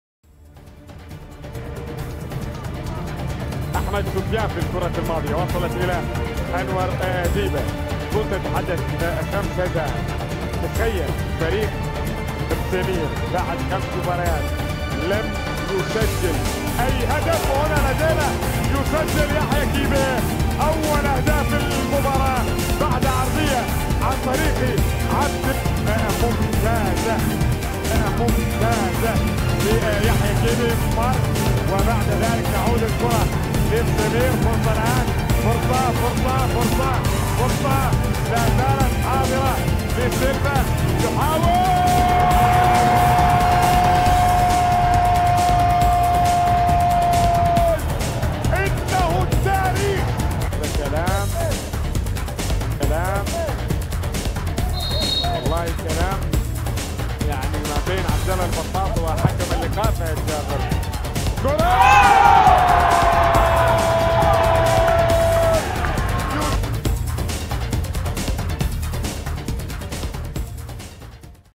Mesaimeer 2 - 1 Al Kharaitiyat (week 6)
اهداف مباراة مسيمير و الخريطيات 2 - 1 من الاسبوع السادس في دوري نجوم قطر 2015/2016

Goals in Mesaimeer 2 - 1 Al Kharaitiyat  match for week 6 in Qatar Stars League 2015/2016